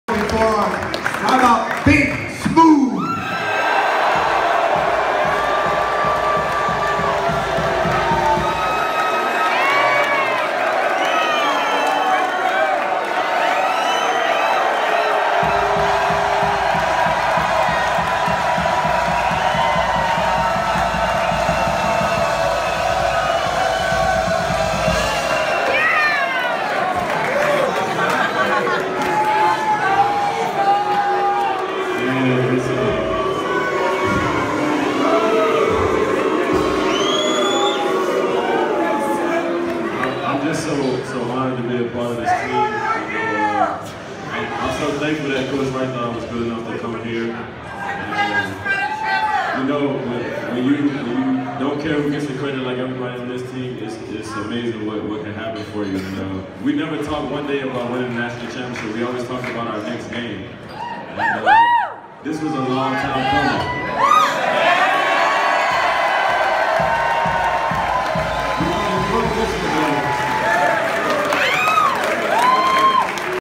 Kris Jenkins at the Villanova 2016 Victory Celebration
At the Royal Sonesta Hotel, Houston, Texas